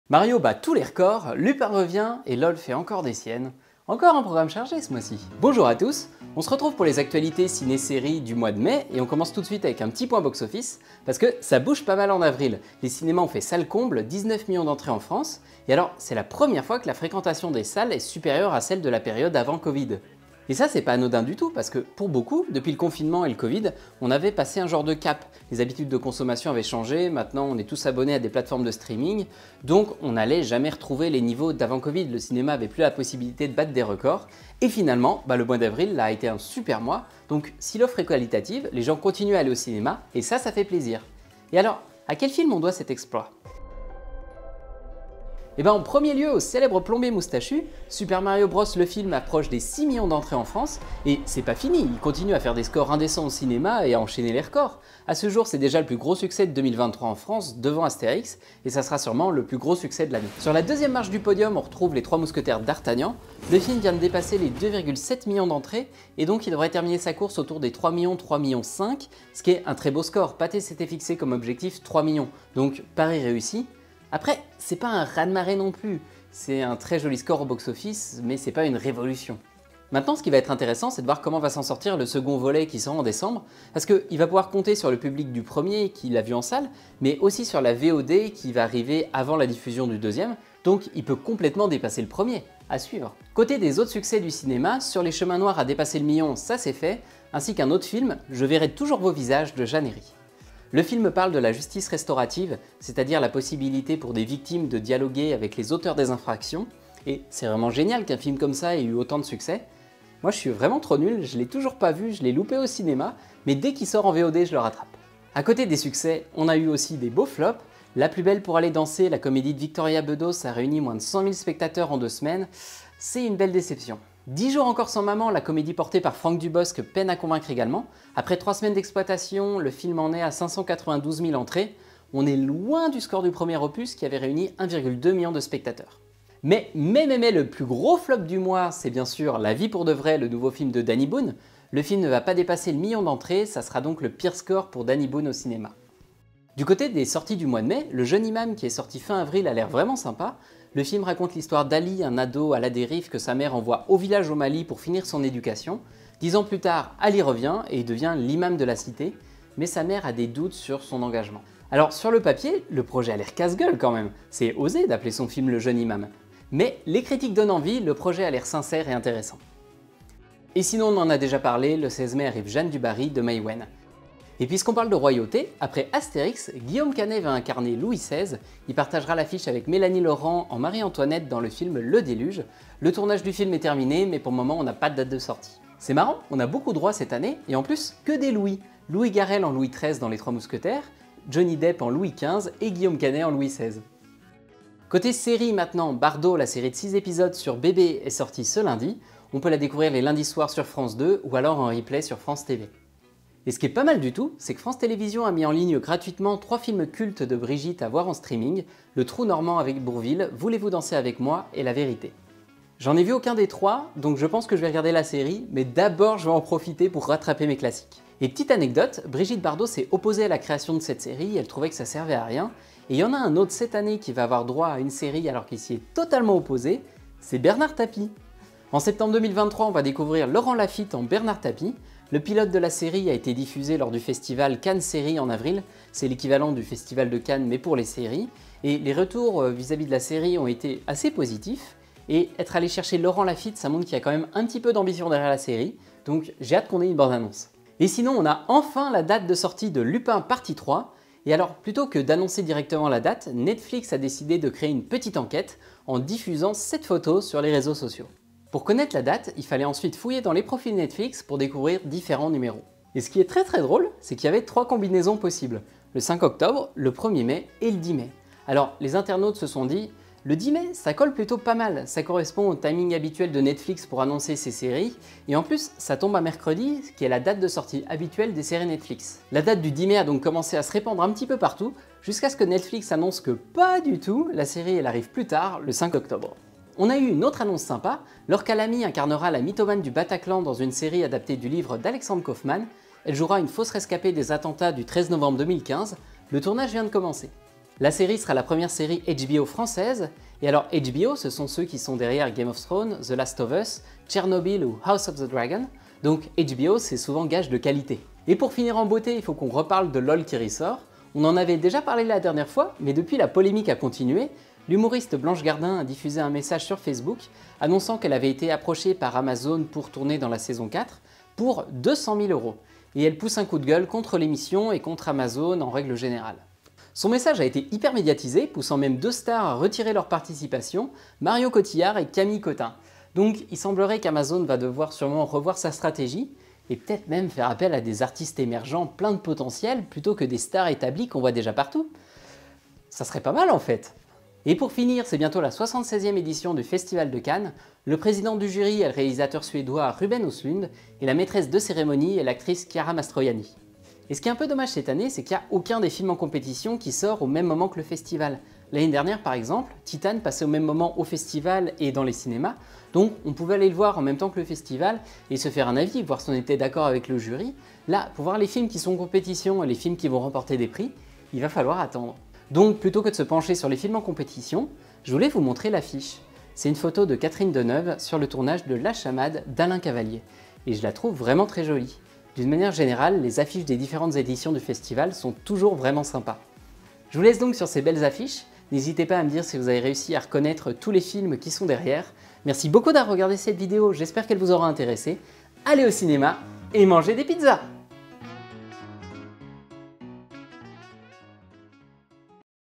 Les cinés font salles combles, Lupin revient et LOL fait n'imp | Les actus ciné de mai
je vous propose de revenir sur les actualités ciné série de mai 2023. Entre le succès de Mario, les nouveautés ciné séries, et le démarrage prochain du Festival de Cannes l'actu est chargé !

Déroulé de la vidéo :
00:00 Actus cinéma
03: 15 Actus séries